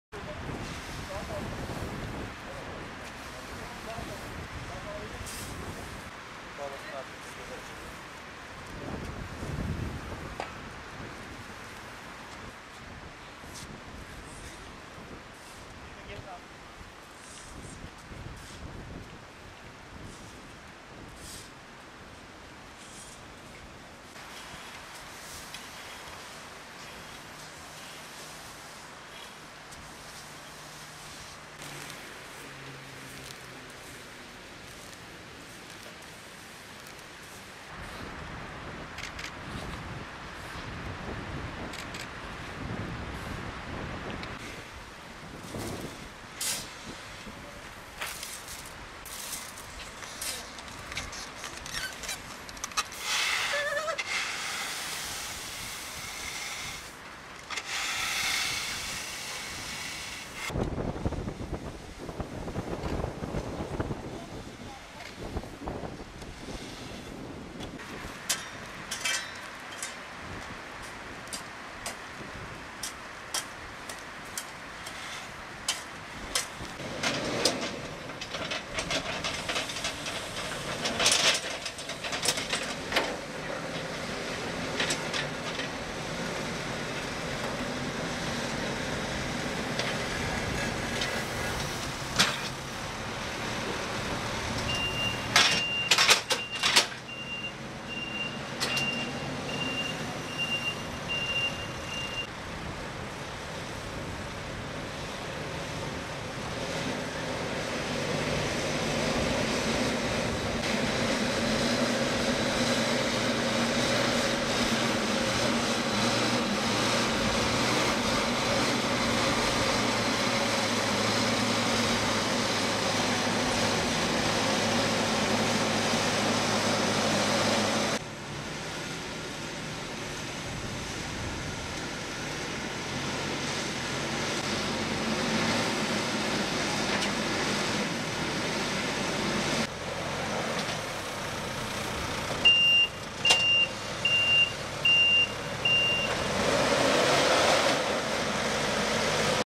BÜYÜKŞEHİR, KOŞU YOLUNDA RUTİN BAKIM ÇALIŞMASI YAPIYOR
Mersin Büyükşehir Belediyesi Park ve Bahçeler Dairesi ekipleri, yurttaşların Adnan Menderes Sahili’nde koşu ve yürüyüş aktivitelerini gerçekleştirdiği Kültür Park sınırları içinde yer alan koşu yolunun rutin bakım ve onarımını gerçekleştiriyor. Ekipler, yoğun kullanım sonucu yıpranan ve çökme oluşan zeminde yeniden kiremit irmiği dökümü gerçekleştirdi.